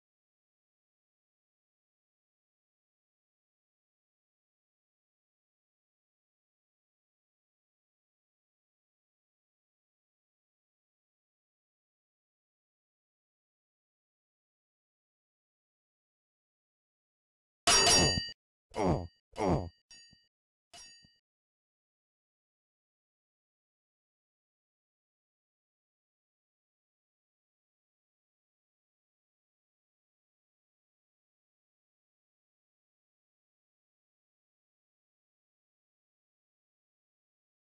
The Game I'm Making!
A quick trailer showing some of the features of the game I'm currently working on. It's a top-down, Zelda-like, adventure game. This is the first build of the project.